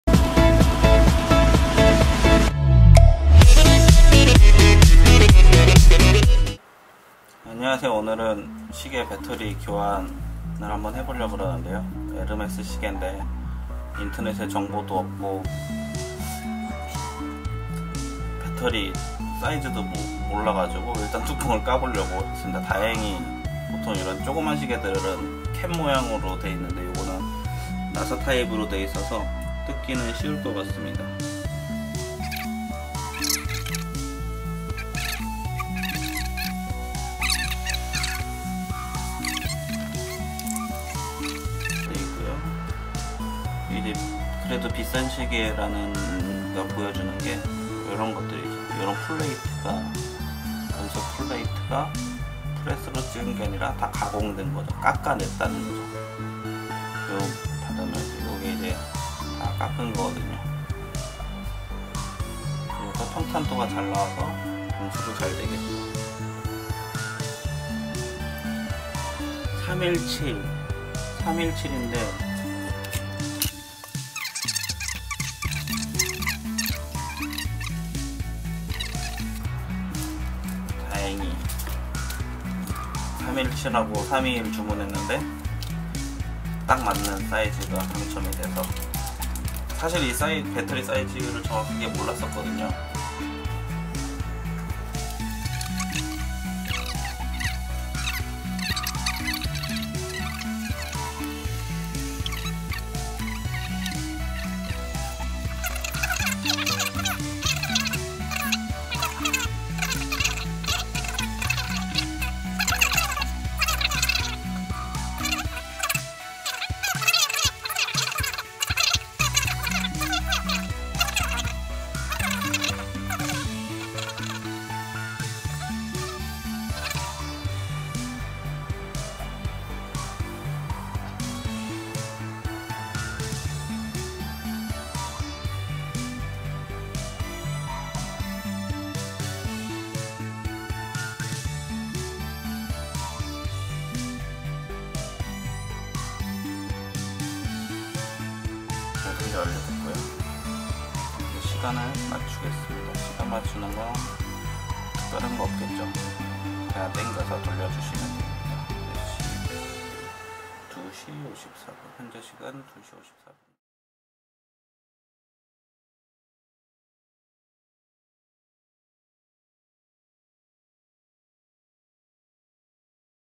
[병달TV] 에르메스 시계 분해해서 배터리 교환하기 hermes
에르메스시계
매장에 맡기지 말고 직접 배터리 교체해보자!
hermes watch
배터리교환

created by BDTV F5